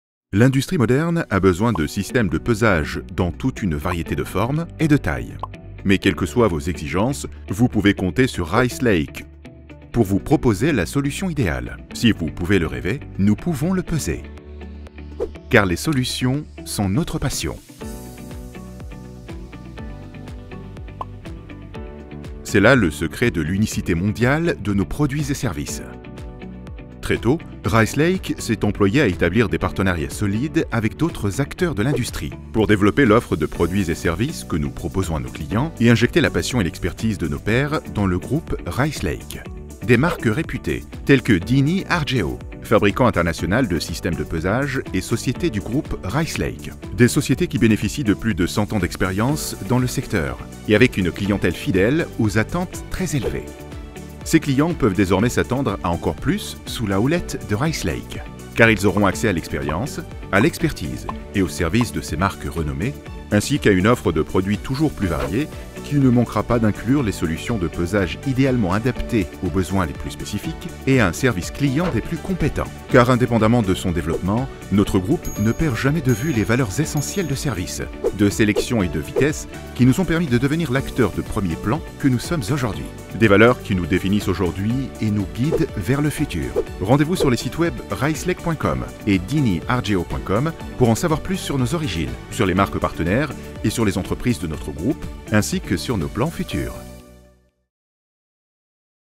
Rice Lake Weighing Systems et Dini Argeo : Solutions de pesage pour le monde entier
Rice Lake Weighing Systems est une entreprise familiale, leader mondial dans le domaine des équipements de pesage, des technologies de contrôle de procédé relatif au poids et des services métrologiques. En 2016, Rice Lake a acheté Dini Argeo, une entreprise italienne spécialisée dans la fabrication et la distribution d’équipements de pesage. Aujourd’hui, la ville de Modène en Italie est la plate-forme de production de Rice Lake en Europe. Grâce à l’offre et à la distribution collectives de leurs produits, les différentes marques Rice Lake sont en mesure de mettre au point des solutions de pesage d’excellente qualité et d’assurer la prestation de leur service client dans le monde entier.